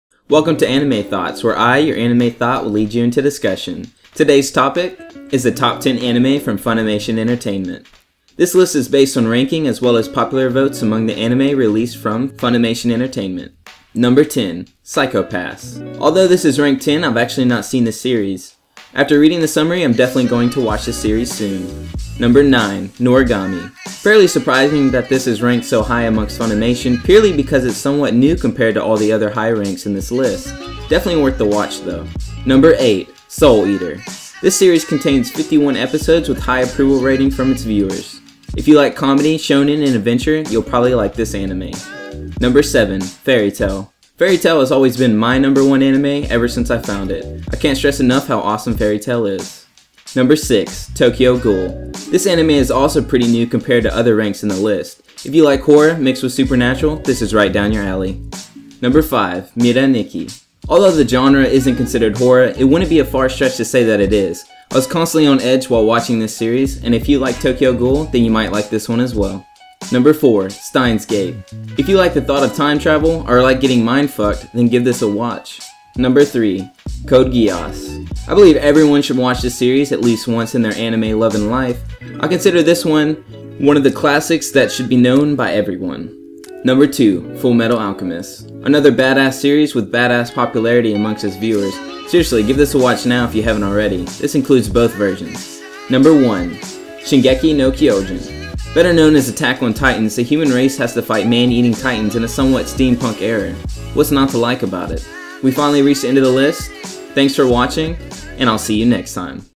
TOP 10 ANIME FROM FUNIMATION OF ALL TIME!?!
Top 10 popular anime from funimation entertainment
This list is based on popularity overall from 2017 to the creation of anime
I was amazed by the ranking myself...